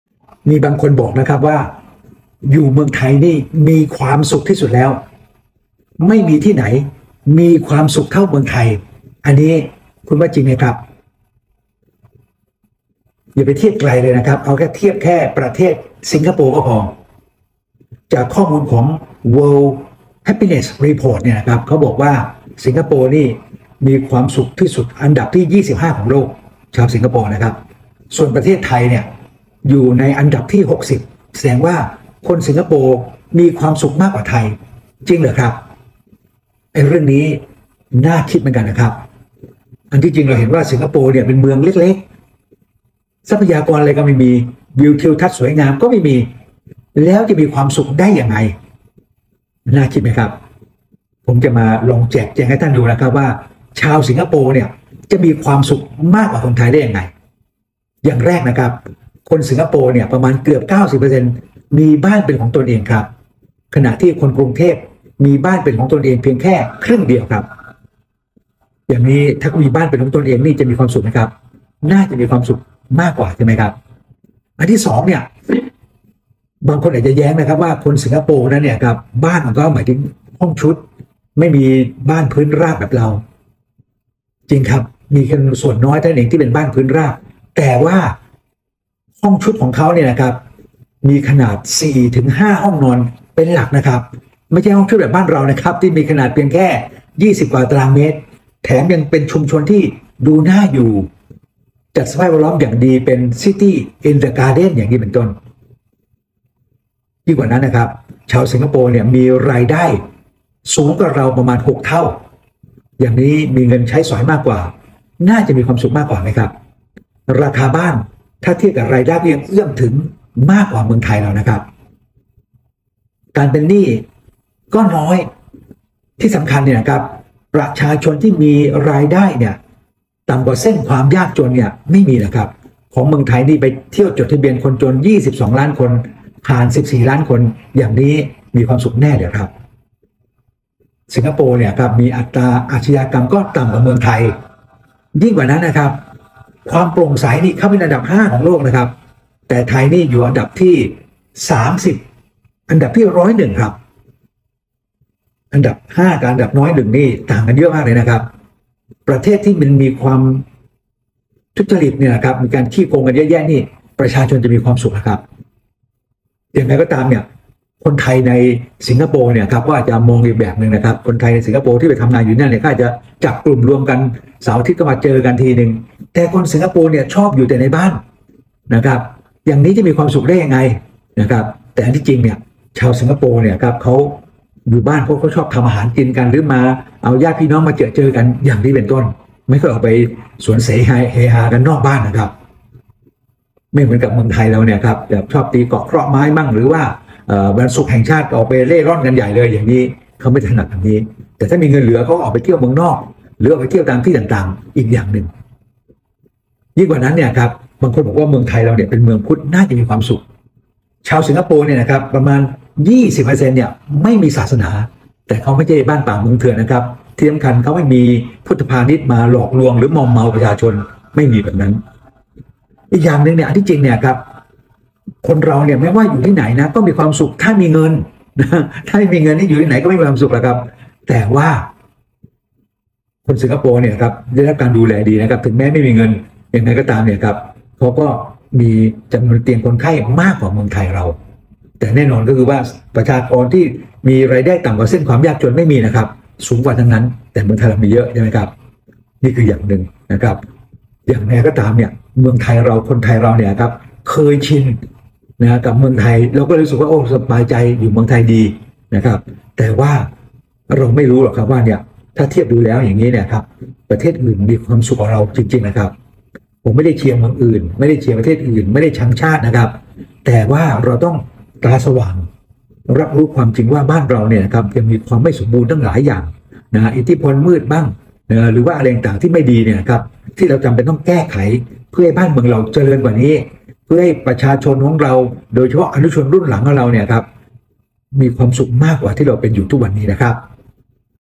อยู่เมืองไทยมีความสุขที่สุด จริงหรือ
เรามักจะได้ยินว่า อยู่เมืองไทยมีความสุขที่สุด นี่เป็นความจริงหรือไม่ ดร.โสภณ พรโชคชัย ประะธานศูนย์ข้อมูลวิจัยและประเมินค่าอสังหาริมทรัพย์ไทย บจก.เอเจนซี่ ฟอร์ เรียลเอสเตท แอฟแฟร์ส ทำคลิปนี้เพื่อเผยแพร่ความคิดเห็นเพื่อพัฒนาชาติ